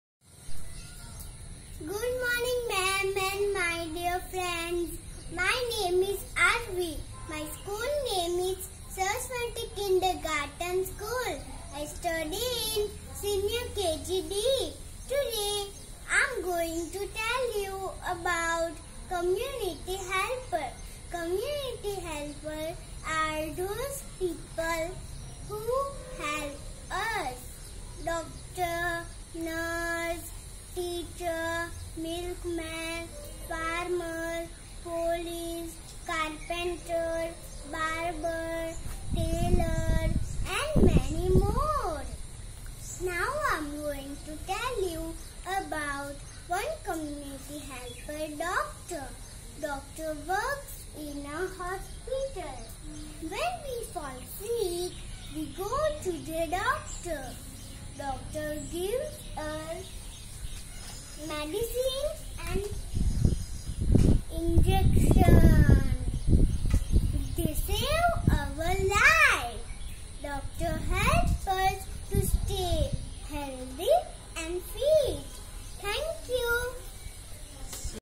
Show and tell on community helper Doctor || kindergarten || Sr.kg
Show and tell on one of the community helper doctor. Sr.kg activities . 17th july 2021